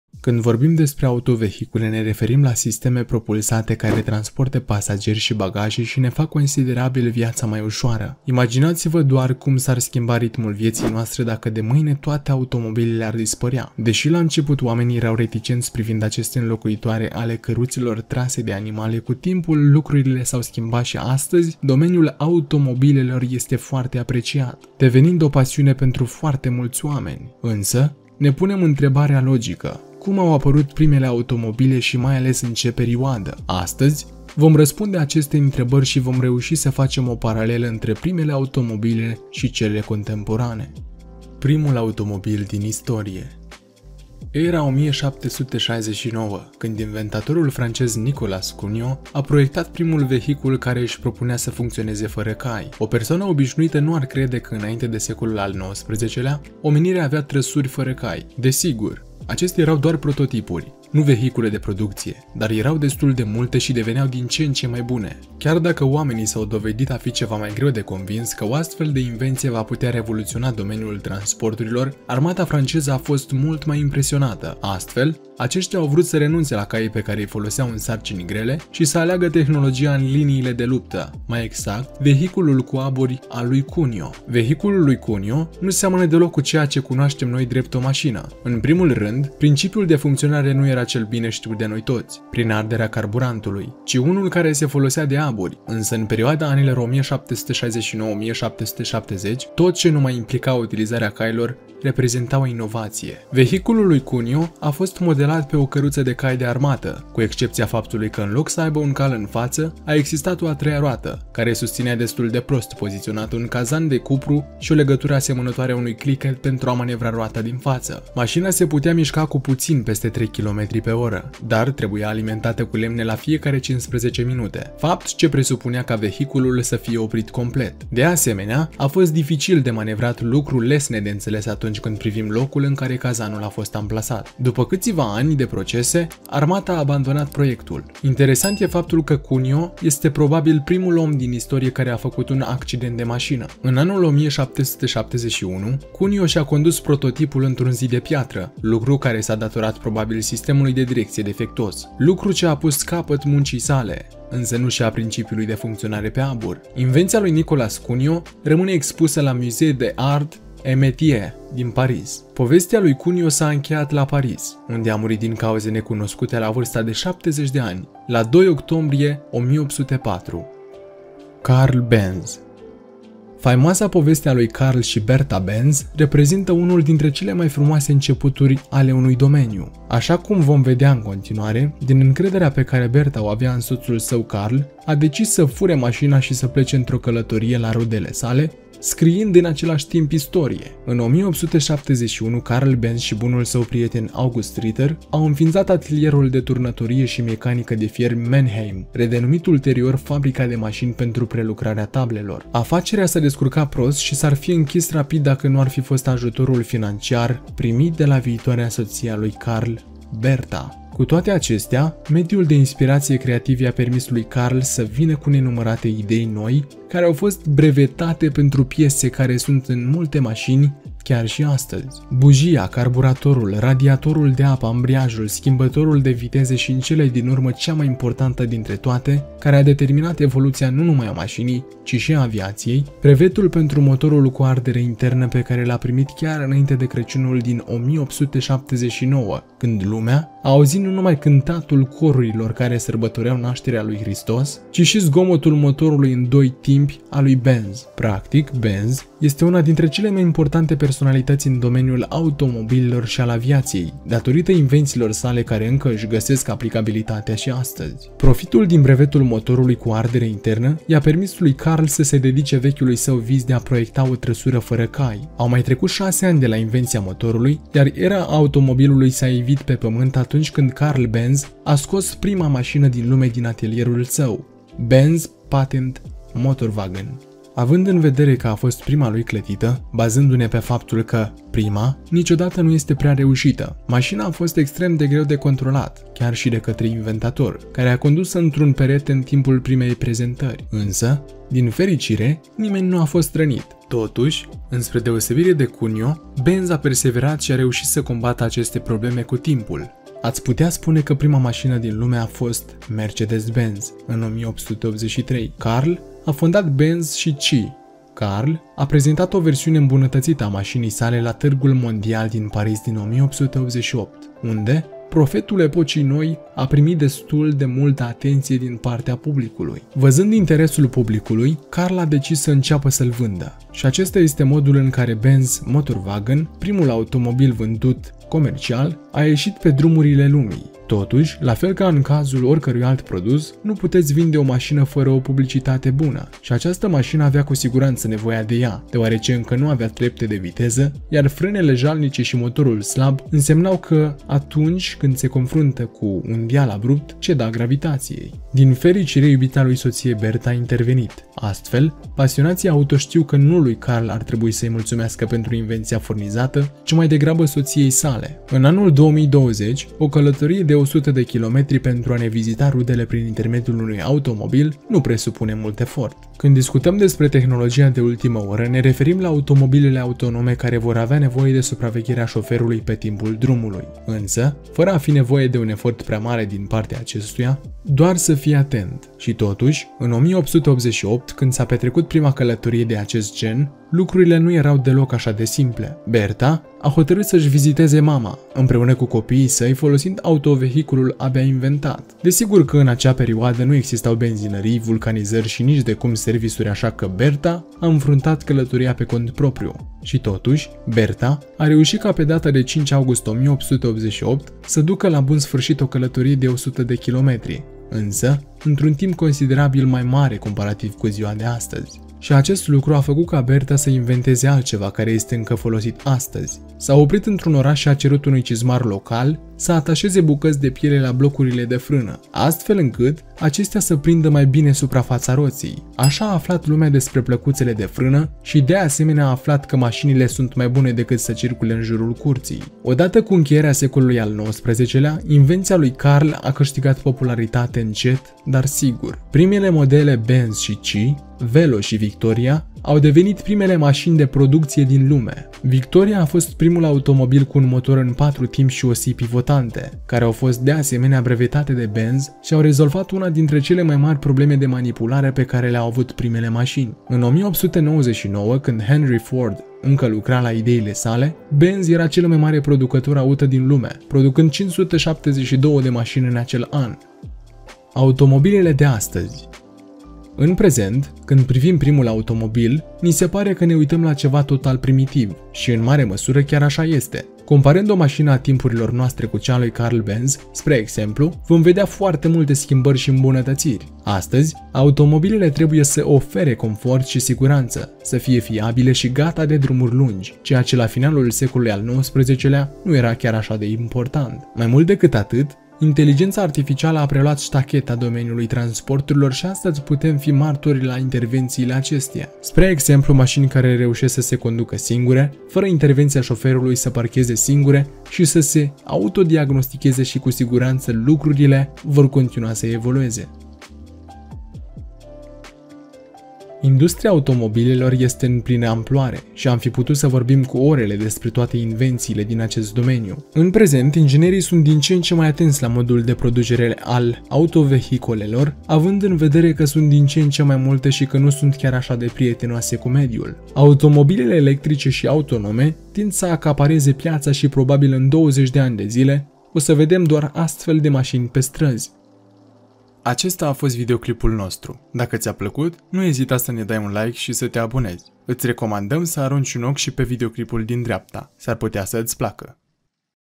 Cum Au Fost Inventate Masinile | Istoria Automobilelor
0:00 - Introducere
0:40 - Primul automobil din istorie
2:50 - Karl Benz
7:47 - Automobilele de astăzi
8:34 - Concluzie